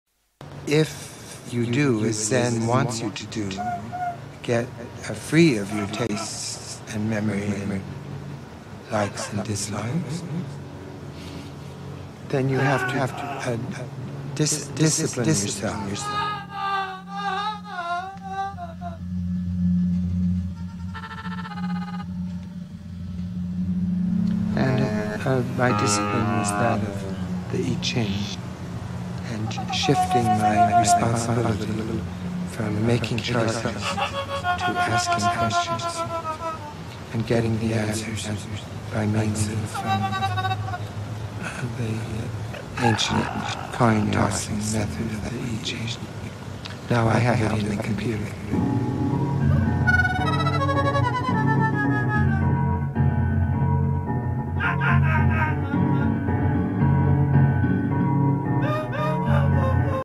Stretching and weaving screams and cage
The final testing and quality control of a pedal like the Stretch Weaver, with it's  dual channel and dynamic nature requires a particular setup. The only solution I found was to juxtapose a snippet of a John Cage interview against screams provided by Simon the Magpie. Sometimes you have to do what you have to do.